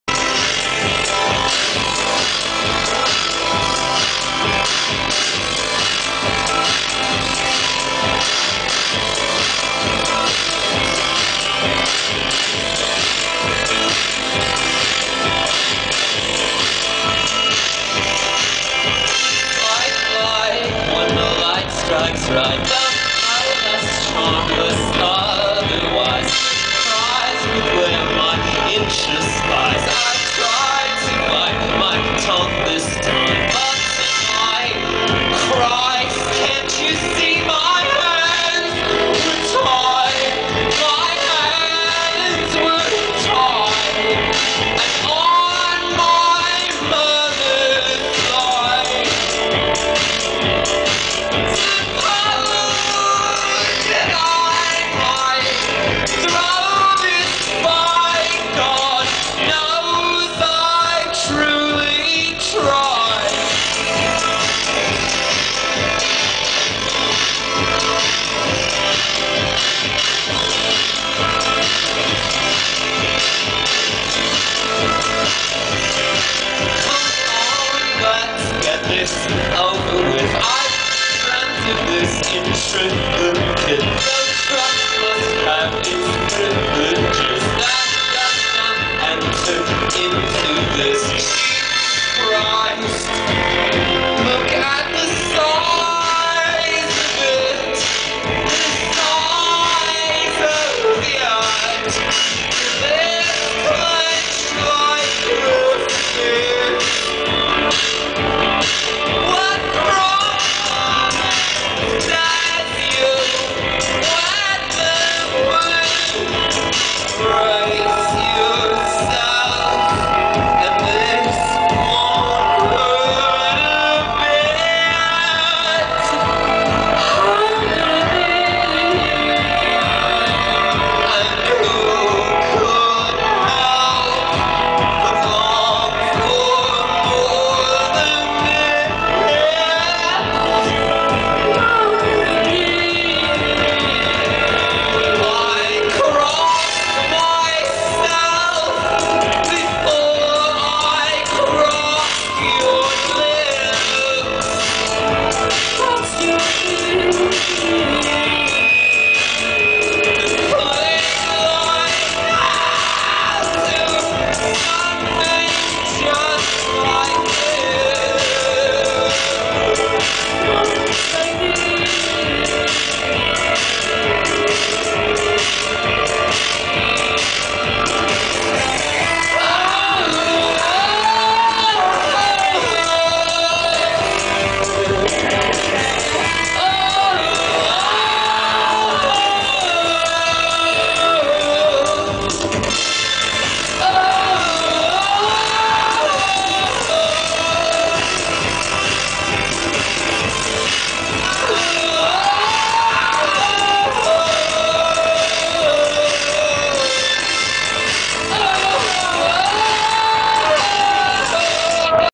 Parenthetical Girls @ Upset The Rhythm (22 May 2010)
Parenthetical Girls play a new song (I didn't catch the name of it...) at Upset The Rhythm at the Luminaire, London on the 22nd May 2010.

I don't normally upload songs that haven't been released but I really like this song...